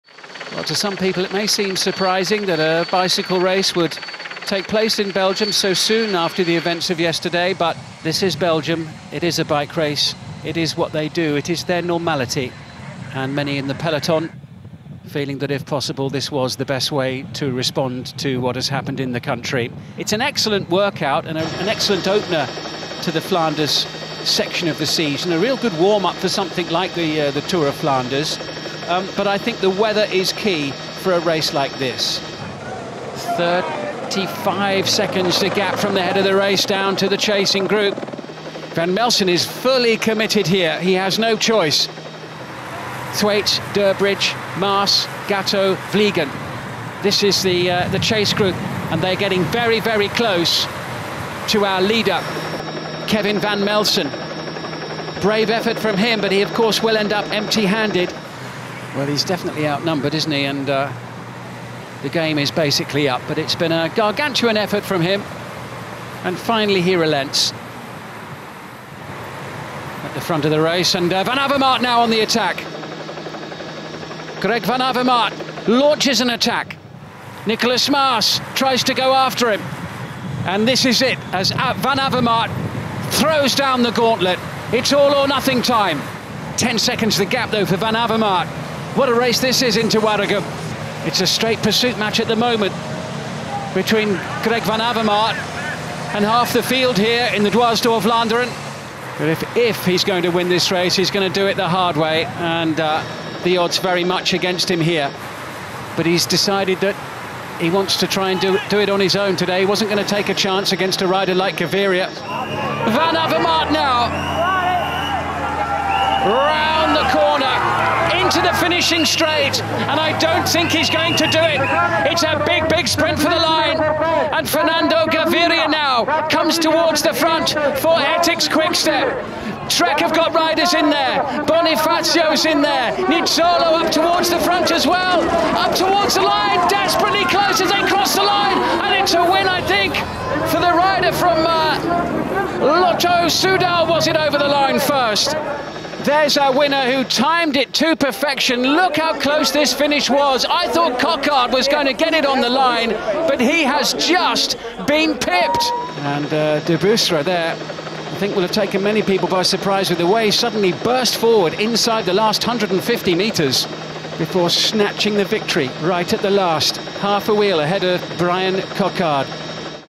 Dwars door Vlaanderen 2016 Highlights
Could home favourite Greg Van Avermaet  take victory ahead of Ettix Quick steps Fernando Gaviria in the 2016 edition of Dwars door Vlaanderen.